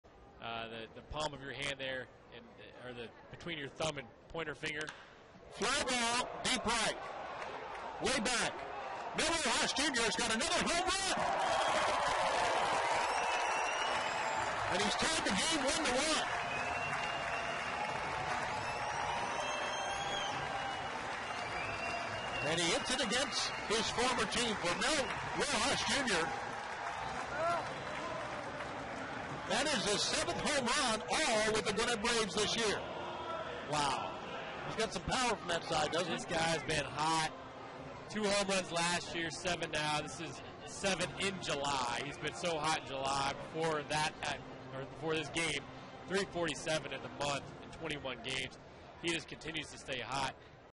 Gwinnett's Rojas Jr. hits homer
7/27/16: Gwinnett right fielder Mel Rojas Jr. ties the game at one with a solo shot